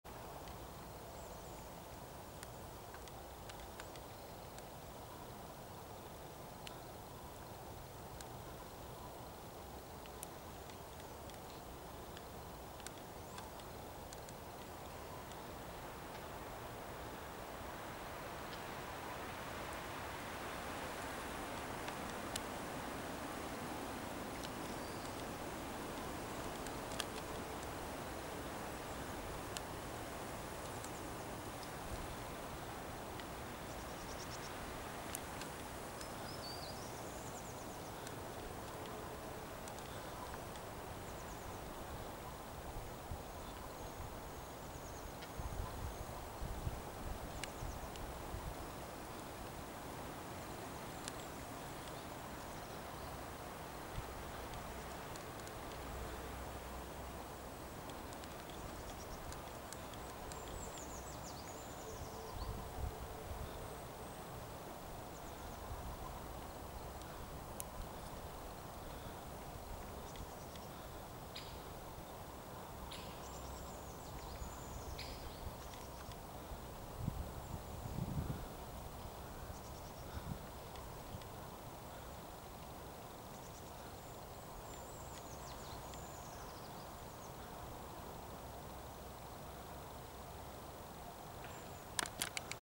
Byczki
chmara byków przechodziła przez drogę .